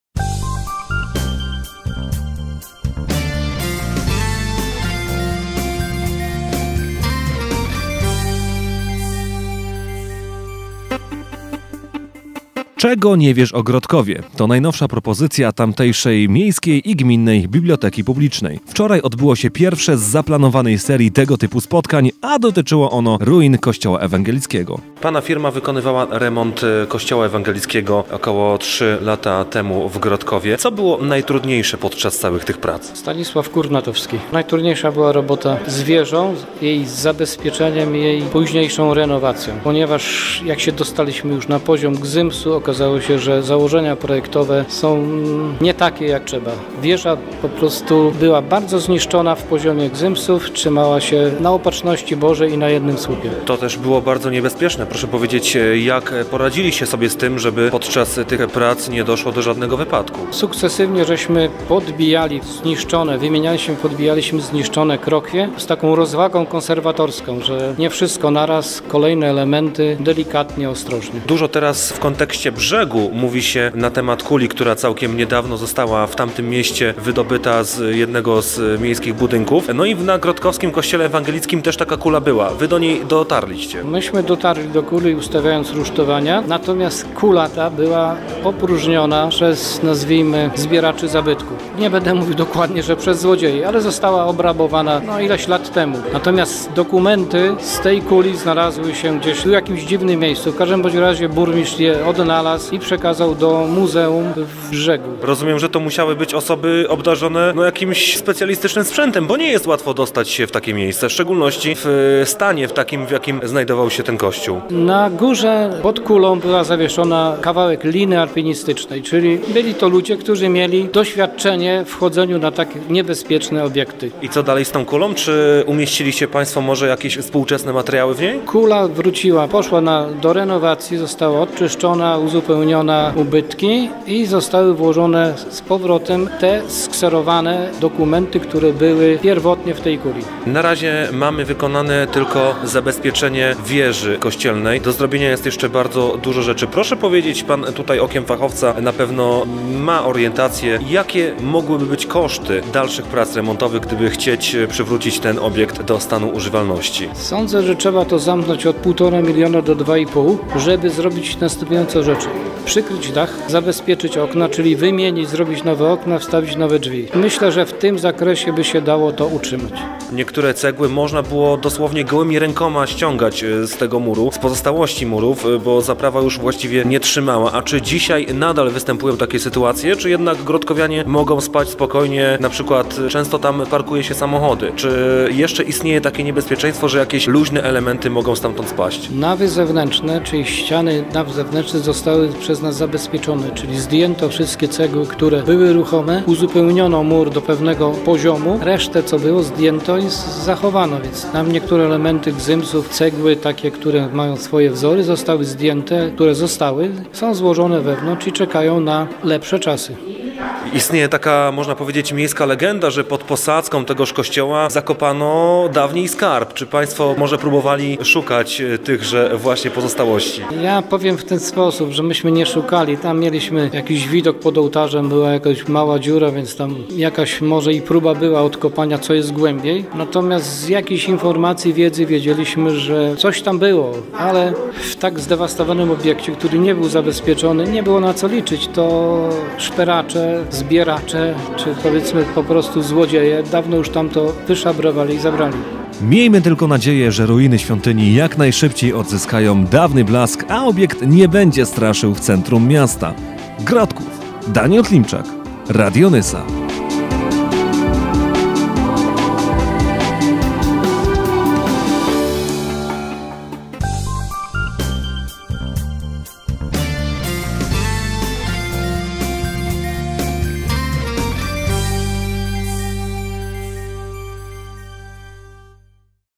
Czego nie wiesz o Grodkowie - Kościół ewangelicki
Grodkowska biblioteka rozpoczęła cykl spotkań "Czego nie wiesz o Grodkowie". Pierwsze z nich poprowadził Stanisław Kurnatowski, który opowiedział o pracach remontowych kościoła ewangelickiego.